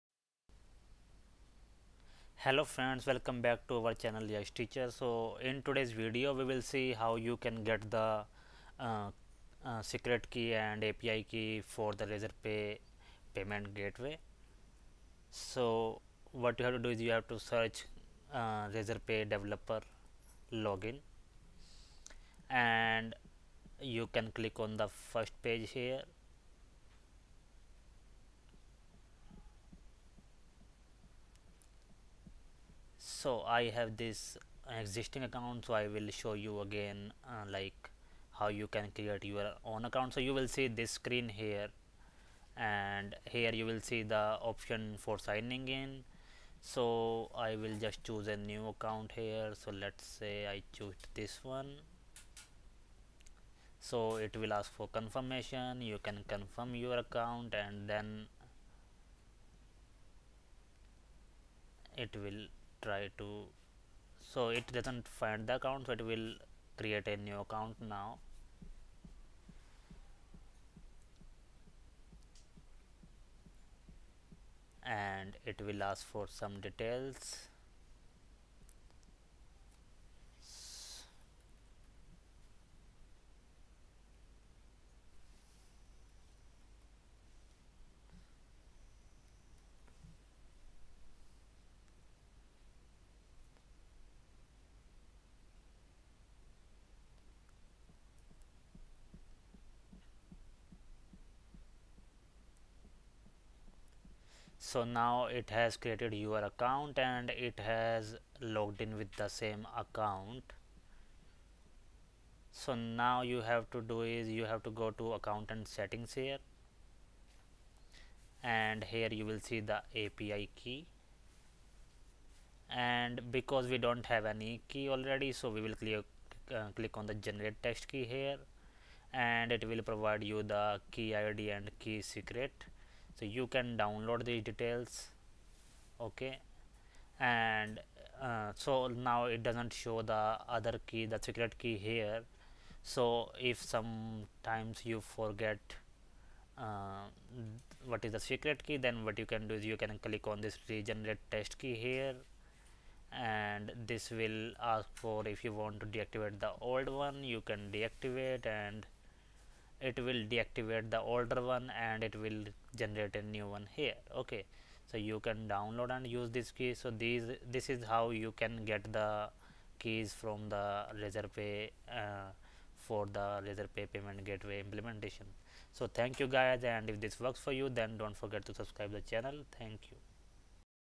How to find api key for RazorPay payment gateway | Razor Pay Api Key
Hello guys in this video we will see how we can get the api key and api secret for implementing razorpay payment gateway.